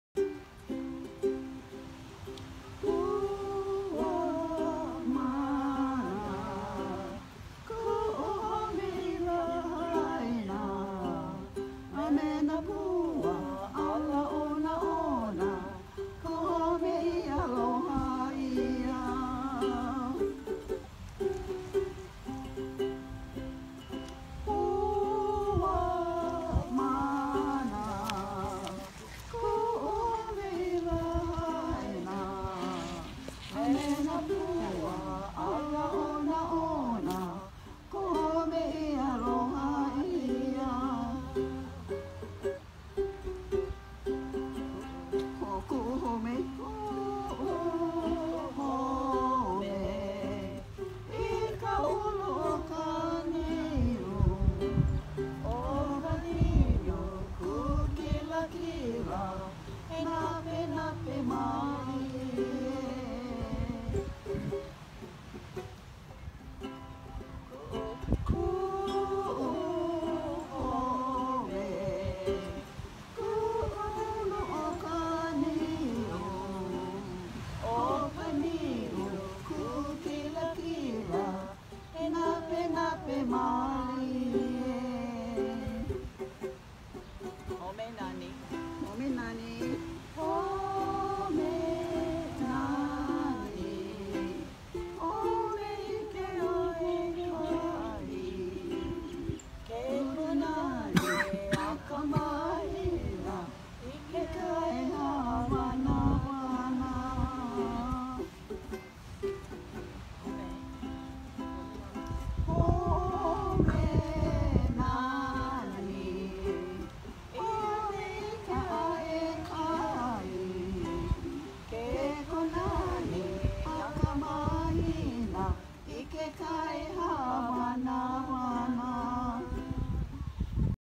Auntie Marion Offering Hula at Beach Mass May 27 2017 mp4
Tonight at our weekly Beach Mass Aunties Mary and Marion offered special music at the start of the service. Aunty Marion offered Hula as Aunty Mary played ukulele accompanied by parishioner Barni Fisher.